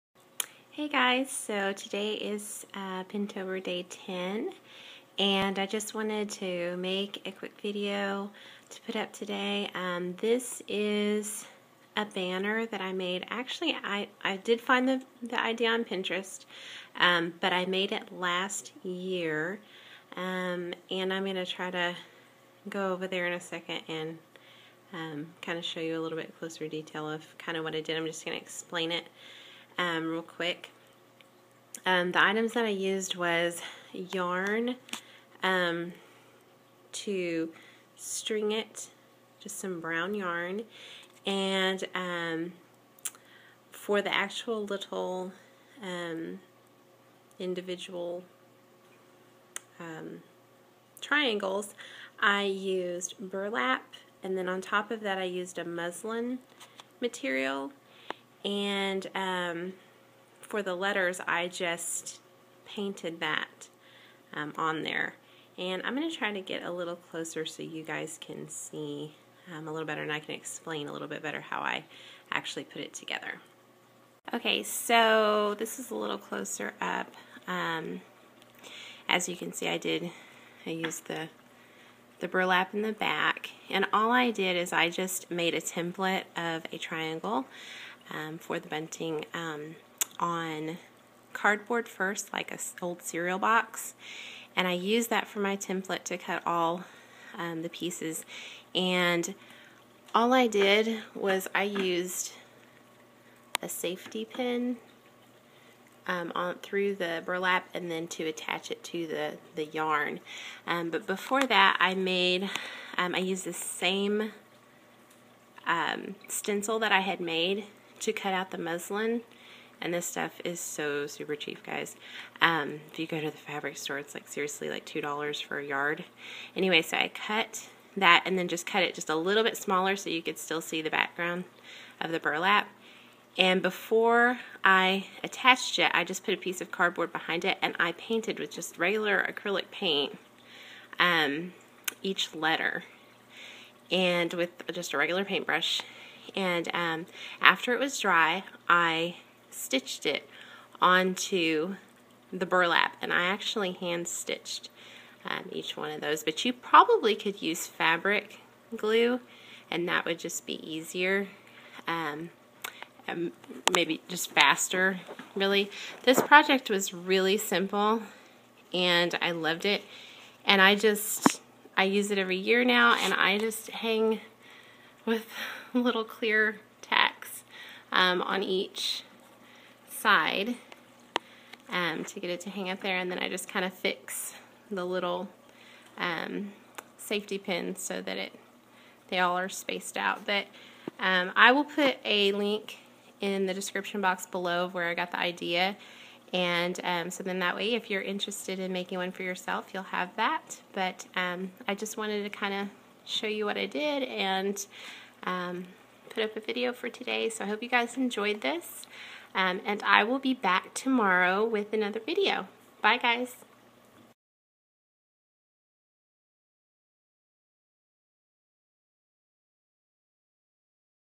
Pintober - Day 10 - Fall Burlap Bunting Banner!
The blog where I originally pinned this from has been deleted :(
However here is a link to the same idea with the same instructions that
the link I had gave me.  When I made mine I did choose to add the Muslin fabric
to paint on because I thought it would make the banner pop and stand out more.